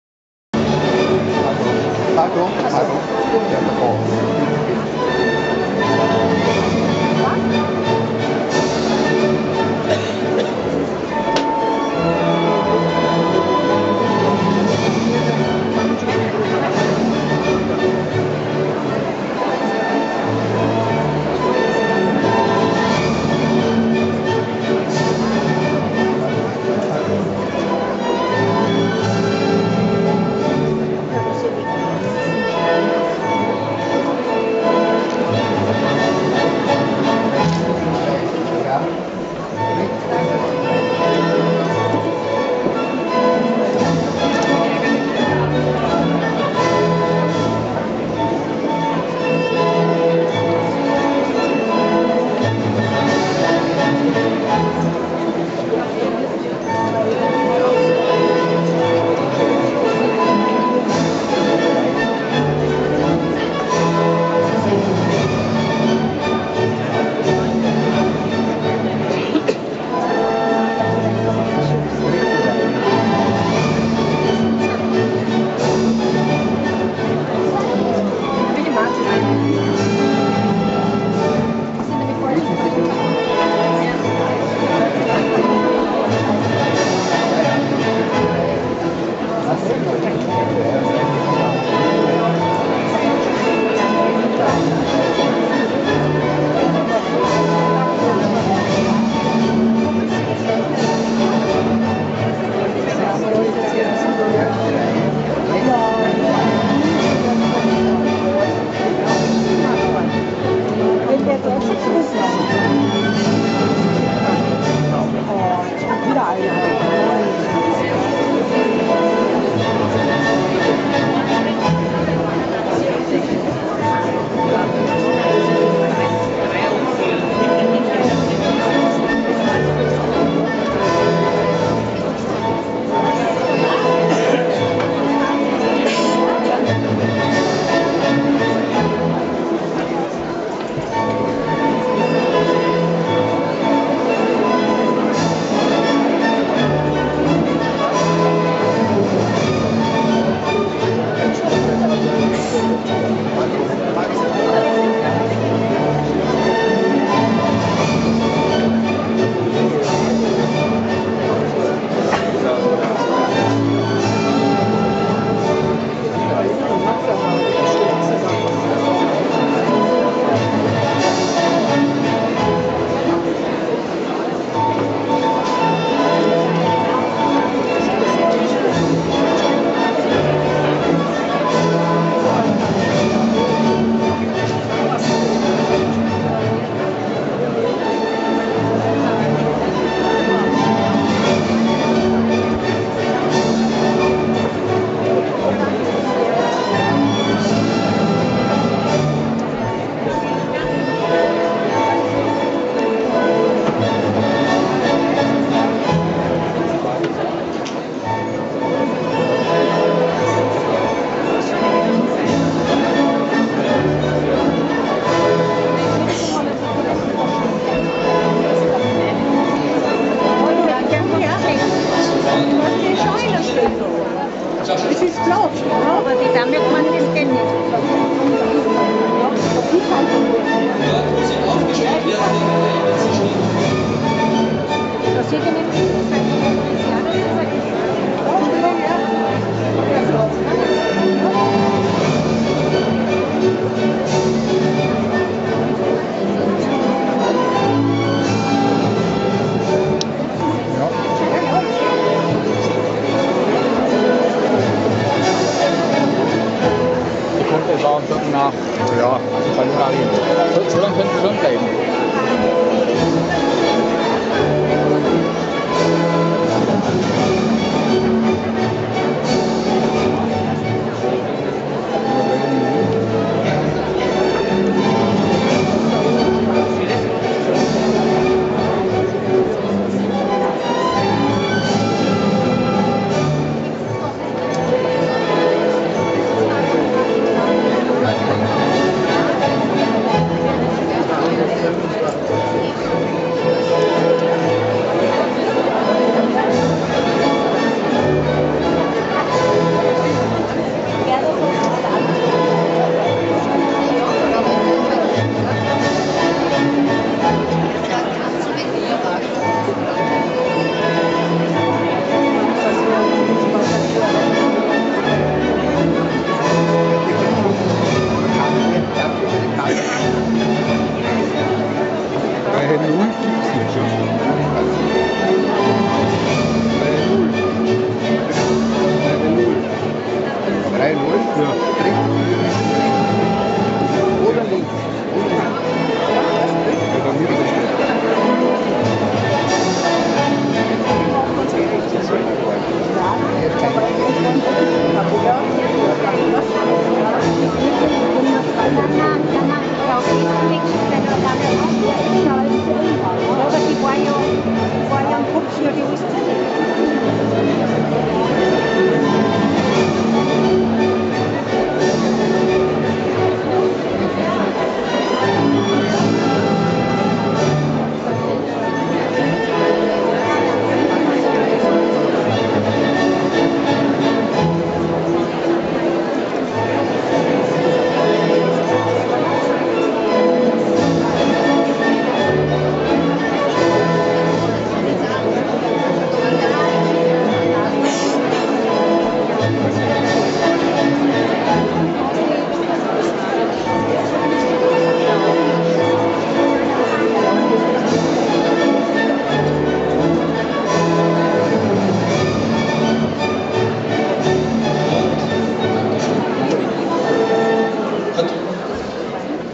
Wiener Opernball Generalprobe 2016, Einzug des Eröffnungskomitees
Einzug des Jungdamen- und Jungherrenkomitees in den Festsaal der Wiener Hofburg beim Opernball.
Bei der Generalprobe zum Wiener Opernball wird jeder Teil der Balleröffnung z.T. mehrmals geübt, damit am Ball auch alles gut ist. Dabei sind jeweils acuh Fortschritte zu sehen. Die Reihen werden gerader, die Figuren präziser. So auch in diesem Jahr.